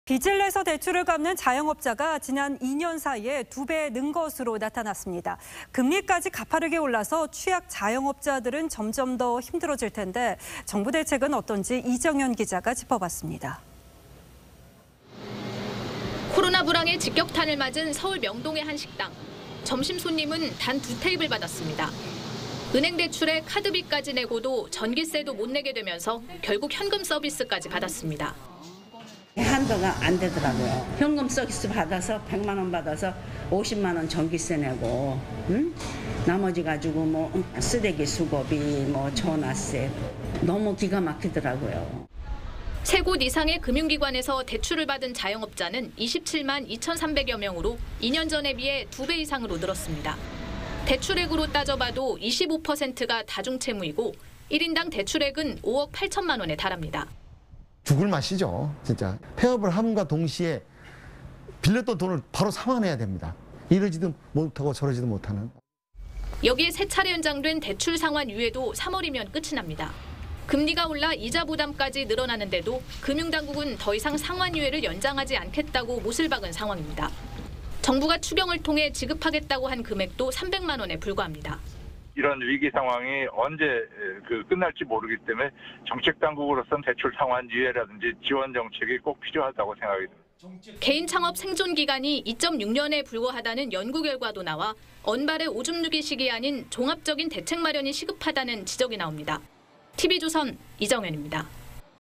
"빚내서 대출 상환"…'3곳 이상 채무' 자영업자 2년새 2배 ↑
빚을 내서 대출을 갚는 자영업자가 지난 2년 사이 2배 는 것으로 나타났습니다. 금리까지 가파르게 올라 취약 자영업자들은 점점 더 힘들어질텐데요.